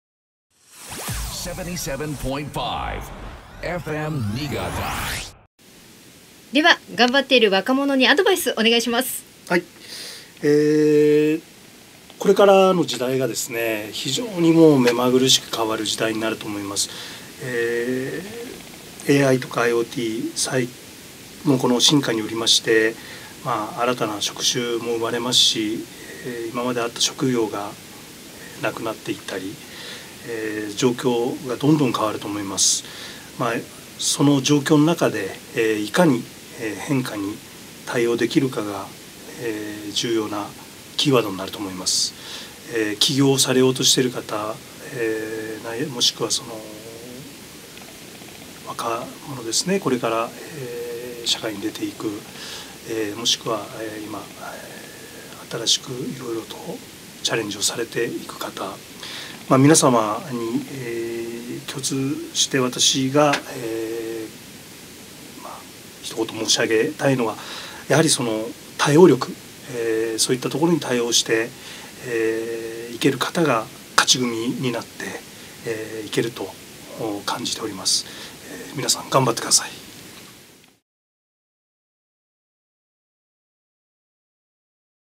(株)ヴァーテックス 帆苅社長から頑張るあなたへ
2018.5/24, 5/31の2回、FM-NIIGATA「CH2020」にご出演いただいた、株式会社ヴァーテックス 取締役社長 COOの帆苅学さんから、「今」を頑張るみなさんへのメッセージをいただきました！